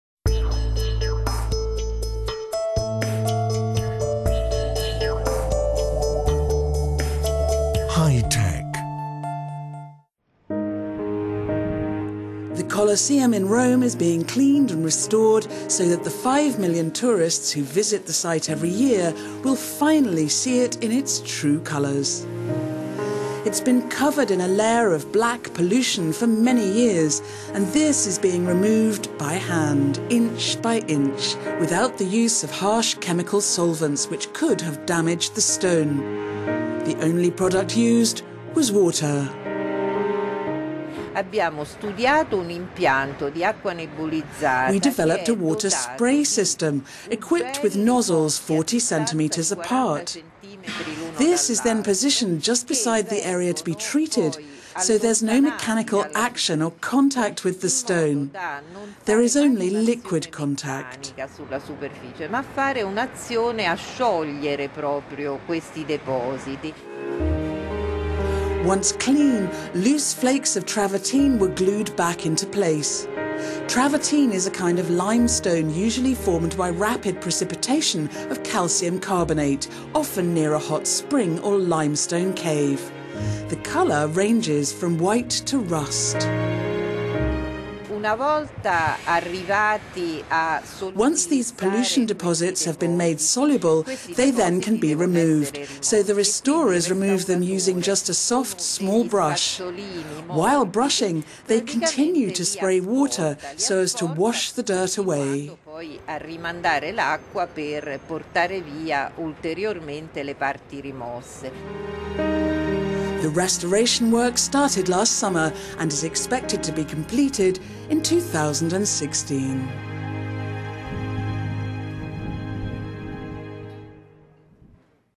Colosseum Recovers Its True Colors - High-Tech
It has been covered in a layer of black pollution for many years, and this has been removed by hand, inch by inch, without the use of harsh chemical solvents which could damage the stone. The only product used is water...